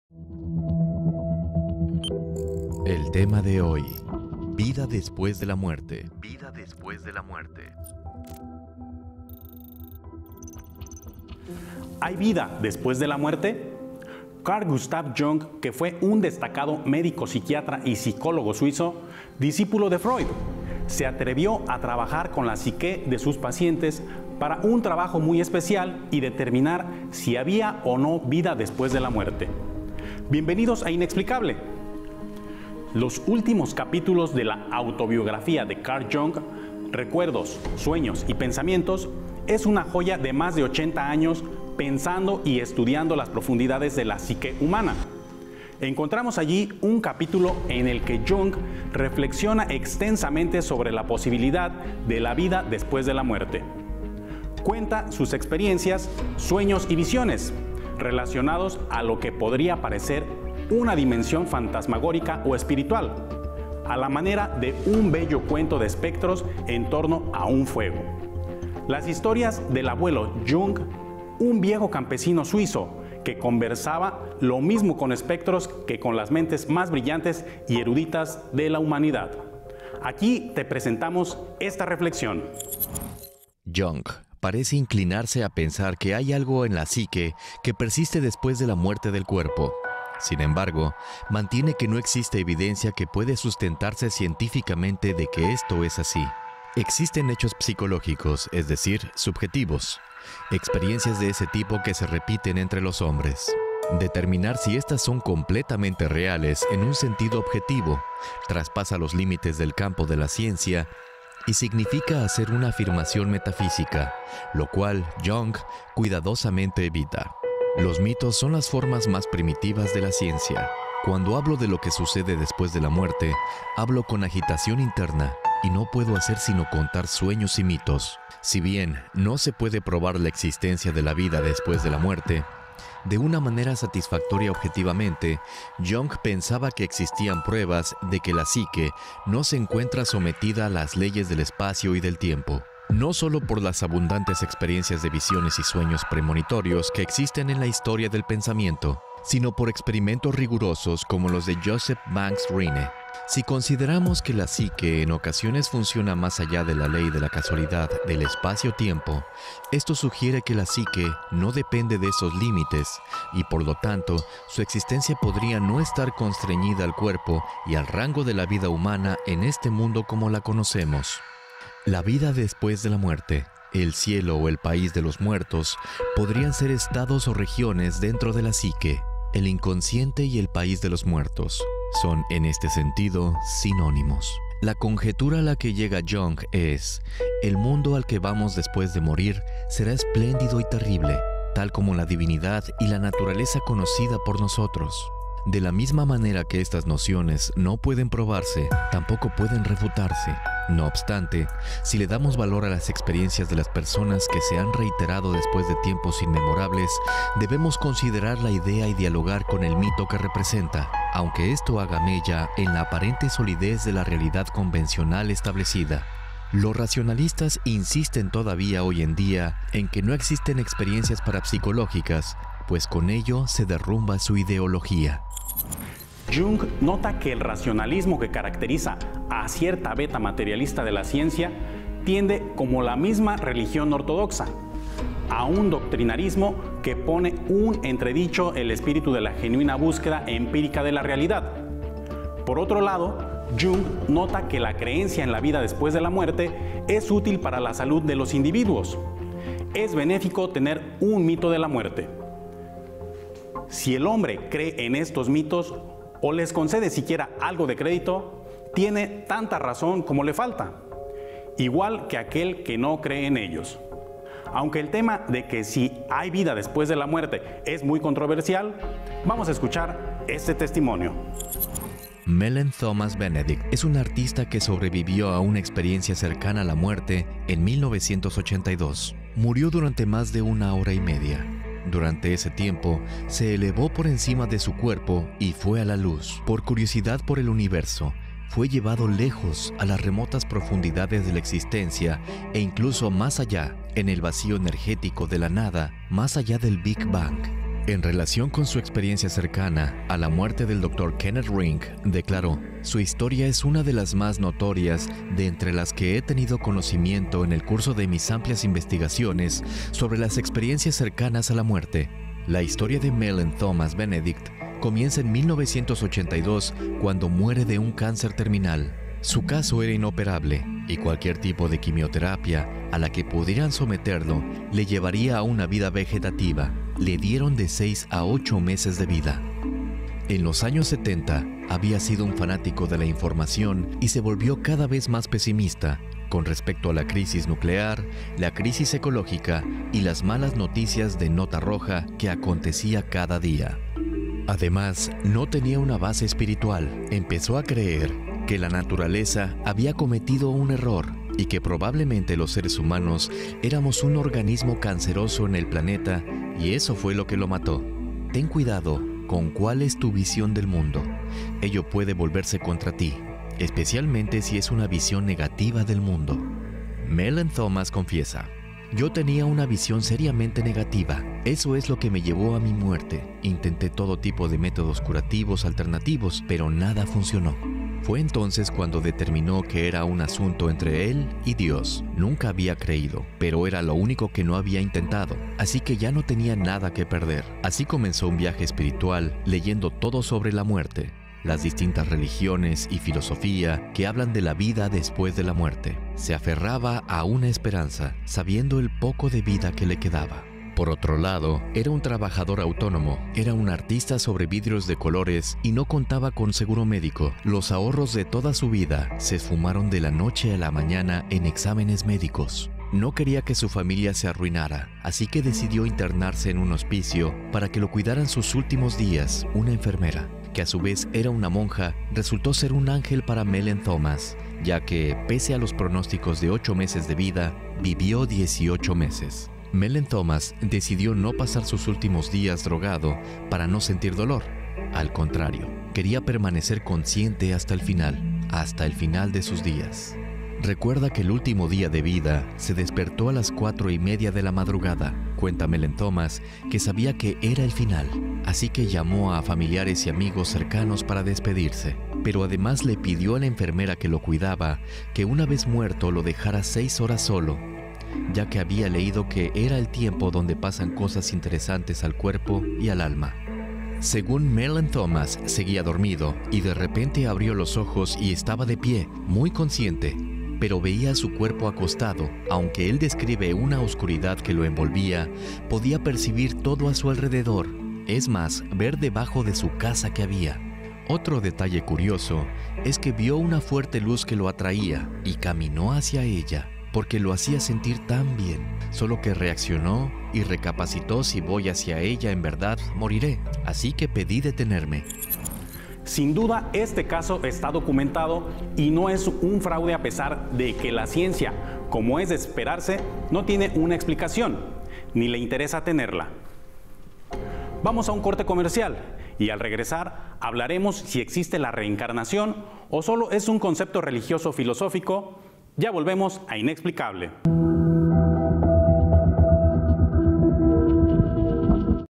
¿HAY VIDA DESPUÉS DE LA MUERTE? PARTE 1 INEXPLICABLE
Ulises nos hace reflexionar sobre los estudios del psicoanalista Carl Jung, donde afirma que no hay que dudar que hay vida detrás de la muerte, es solo la estreches científica que no lo acepta.